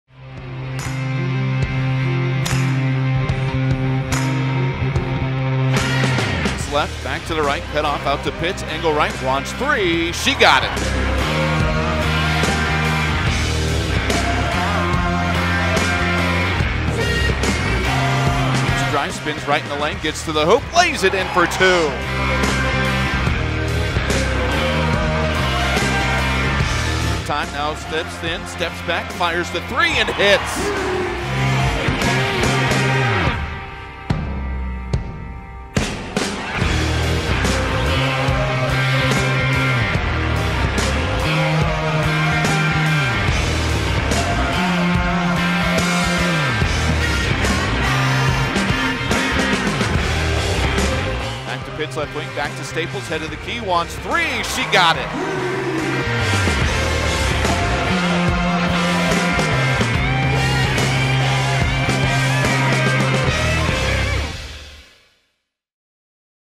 Highlights: Gopher Women's Basketball vs. Missouri State
Gopher Women's Basketball dropped their season opener on Nov. 5 at Williams Arena.